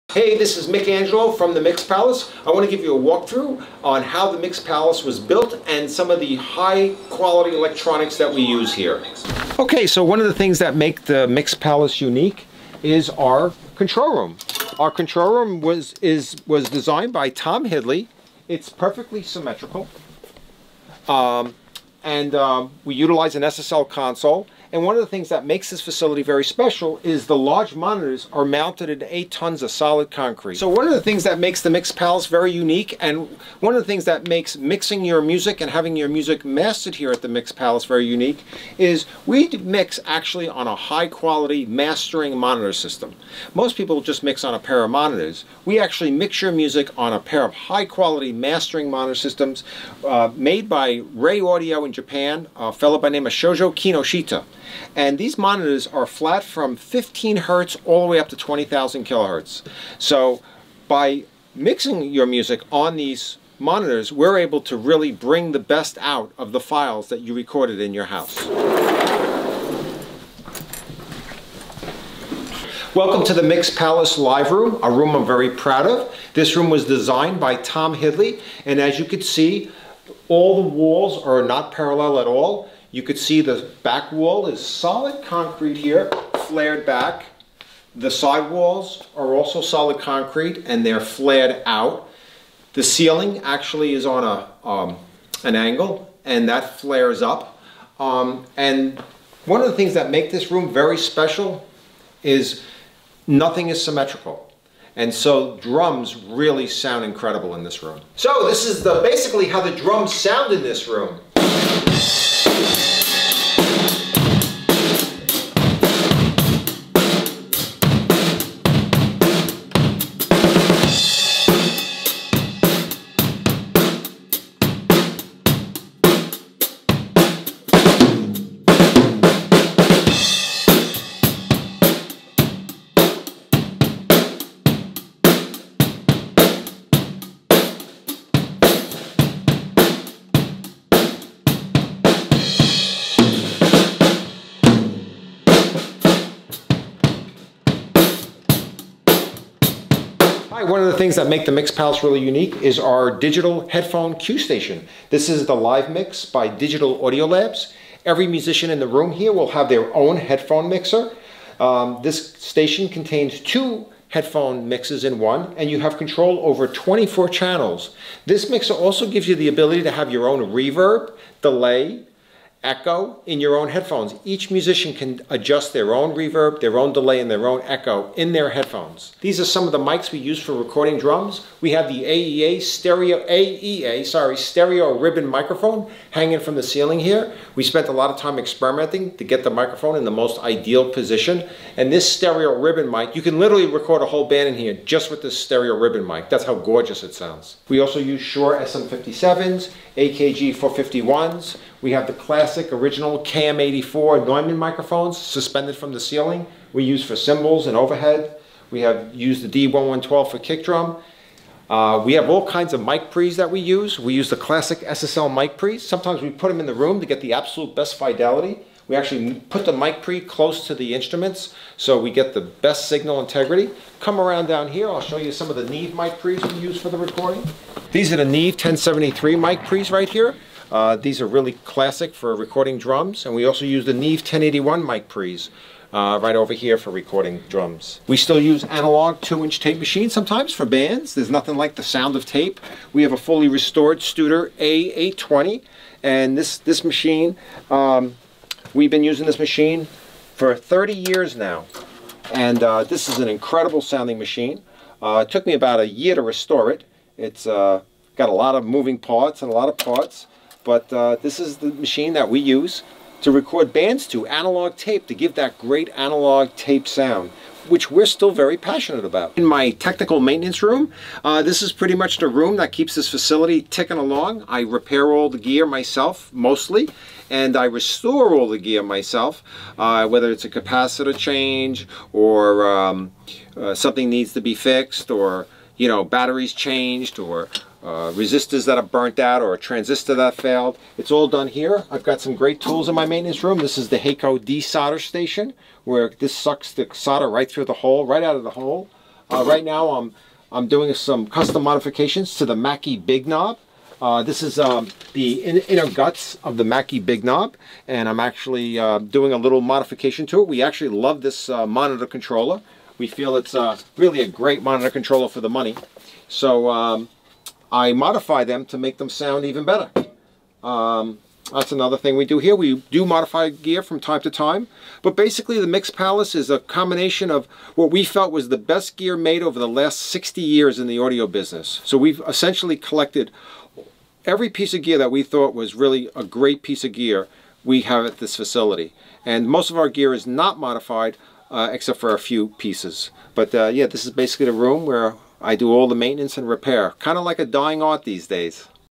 The Mix Palace Walkthrough
Take a quick tour into some of the rooms found at The Mix Palace with the owner MicAngelo